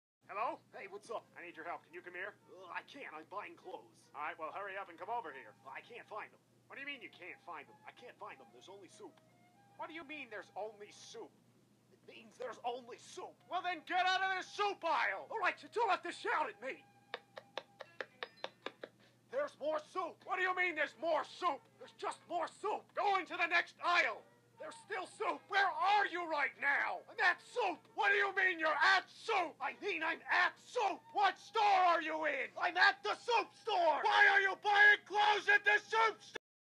Nightcreeper Goes To Soup
I go to buy clothes. And there's also Shark Dude, from MY BUDDIES!